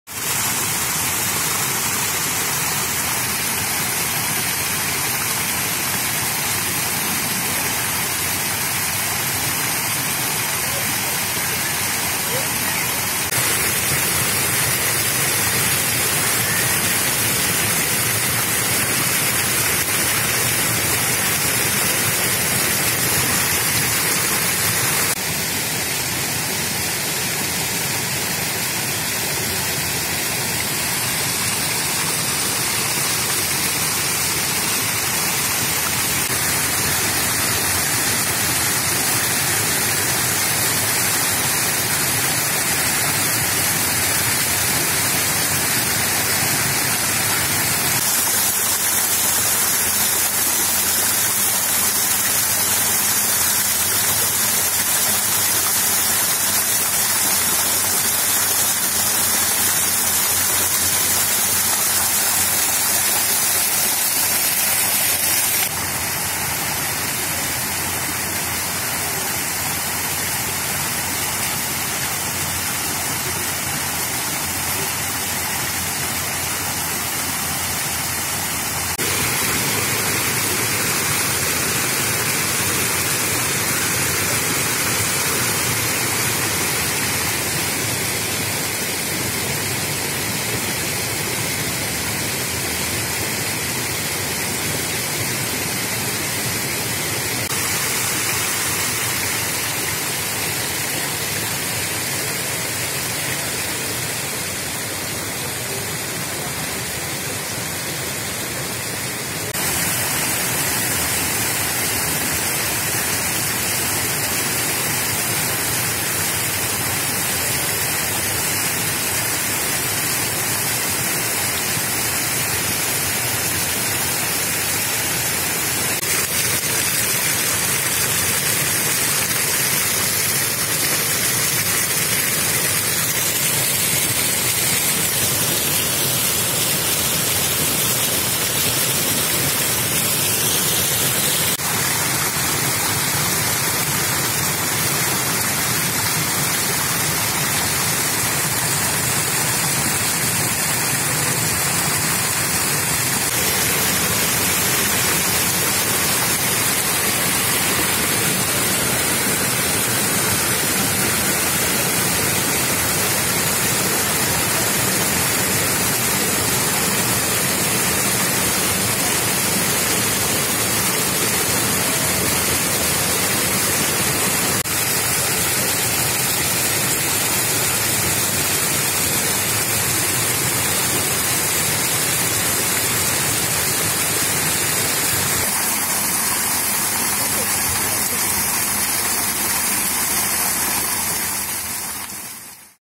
Fuente de Nezahualcóyotl - 4K
Fuente de Nezahualcóyotl en el Bosque de Chapultepec, Ciudad de México.